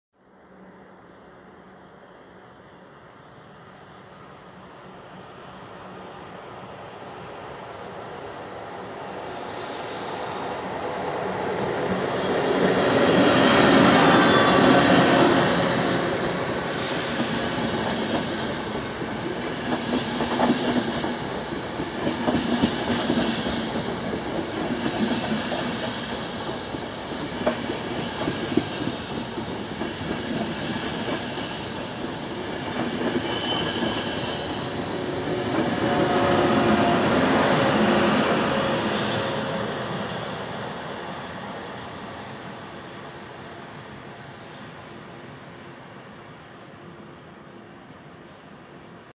Treno Intercity sun,2 e414 dts,Arquata Scrivia,succursale Giovi,trains hunter,railways,trainspotting
19 10 21 ore 22.33

Transito sul binario 5 della stazione di Arquata Scrivia sulla linea succursale dei Giovi,di un Intercity sun con le 2 e414 in doppia trazione simmetrica entrambe in livrea giorno.

e ancora architettura,montagna,trekking,vie ferrate. motori